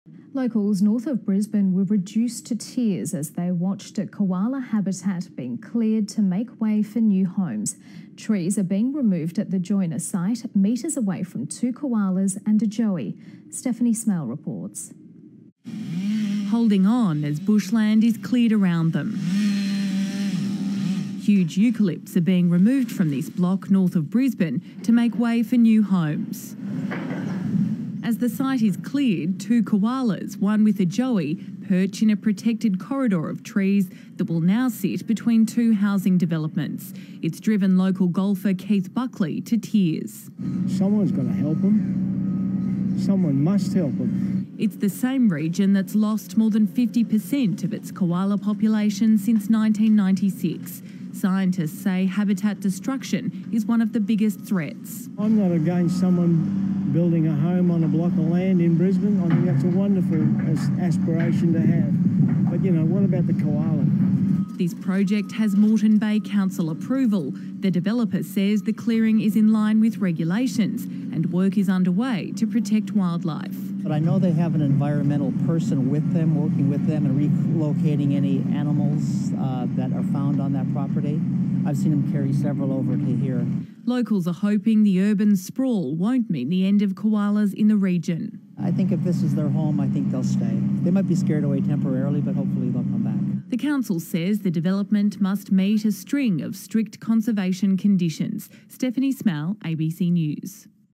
2 koalas & a joey hang on as vegetation is felled around them at Morton Bay
31/05/2016 Two Koalas and a joey hang on as vegetation is cleared around them leaving them with only a narrow strip of trees between two housing developments.